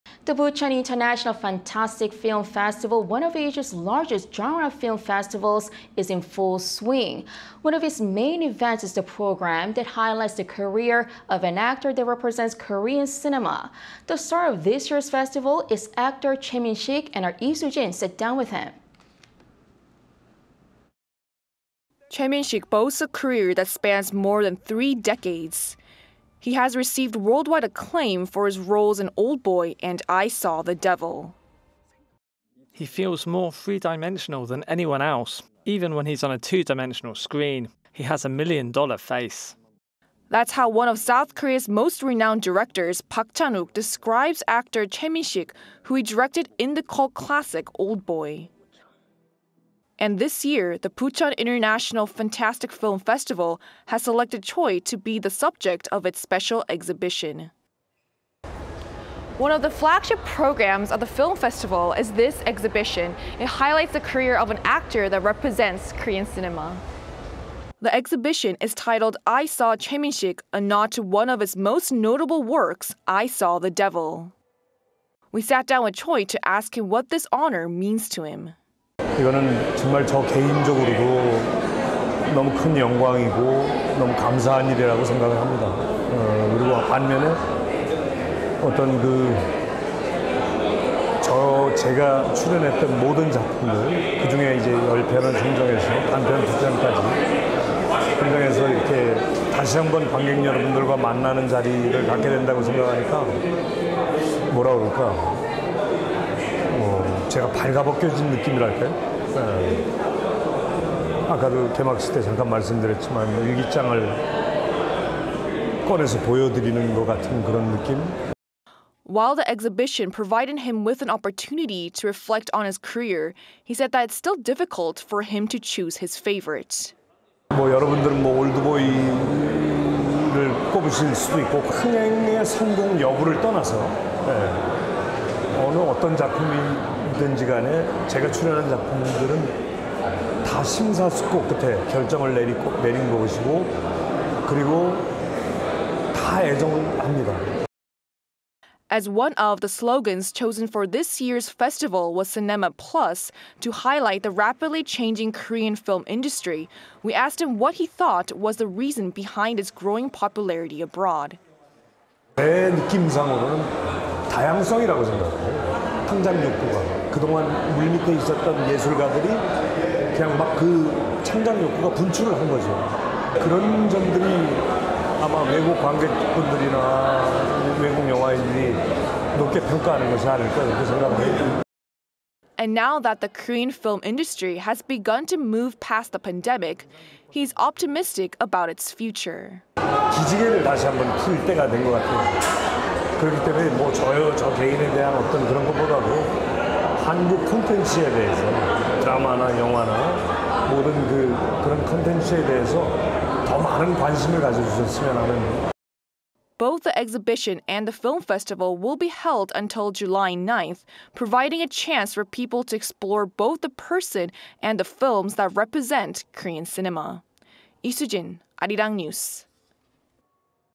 Bucheon International Fantastic Film Festival 2023: Choi Min-sik sits down with Arirang TV to ...
Bucheon International Fantastic Film Festival 2023: Choi Min-sik sits down with Arirang TV to reflect on his career, discuss Korean cinema

제27회 부천국제영화제: 'BIFAN 특별전' 주인공 배우 최민식 인터뷰

The Bucheon International Fantastic Film Festival --one of Asia’s largest genre film festivals --is in full swing.
One of its main events is the program that highlights the career of an actor that represents Korean cinema.
The star of this year's festival is actor  Choi Min-sik.
And our Lee Soo-jin sat down with him.

Choi Min-sik boasts a career that spans more than three decades.
He has received worldwide acclaim for his roles in “Oldboy” and “I Saw the Devil.”

“He feels more three-dimensional than anyone else, even when he‘s on a two-dimensional screen. He has a million-dollar face.”

That’s how one of South Korea's most renowned directors, Park Chan-wook, describes actor Choi Min-sik, who he directed in the cult classic “Oldboy.”
And this year the Bucheon International Fantastic Film Festival has selected Choi to be the subject of its special exhibition.

"One of the flagship programs of the film festival is this exhibition. It highlights the career of an actor that represents Korean cinema."

The exhibition is titled “I Saw Choi Min-sik”, a nod to one of his most notable works “I Saw the Devil.”
We sat down with Choi to ask him what this honor means to him.

While the exhibition provided him with an opportunity to reflect on his career, he said that it's still difficult for him to choose his favorite.

As one of the slogans chosen for this year's festival was “Cinema+” to highlight the rapidly changing Korean film industry, we asked him what he thought was the reason behind its growing popularity abroad.

And now that the Korean film industry has begun to move past the pandemic, he is optimistic about its future.

Both the exhibition and the film festival will be held until July 9, providing a chance for people to explore both the person and the films that represent Korean cinema.
Lee Soo-jin, Arirang News.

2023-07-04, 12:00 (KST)